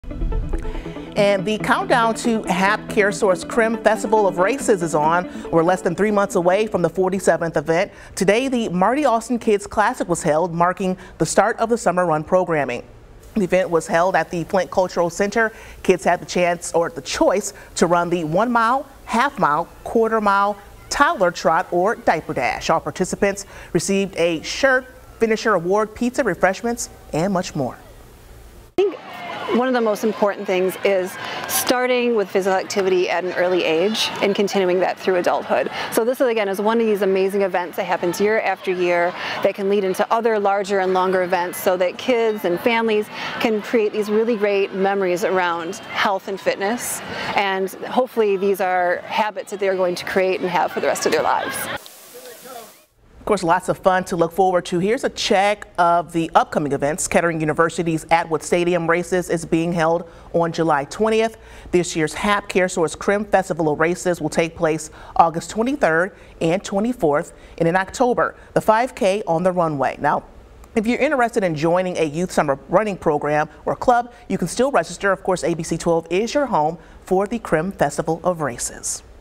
Many kids participated in the Marti Austin Kids' Classic on Saturday
The Marti Austin Kids' Classic was held on Saturday, just a few months ahead of the HAP CareSource Crim Festival of Races.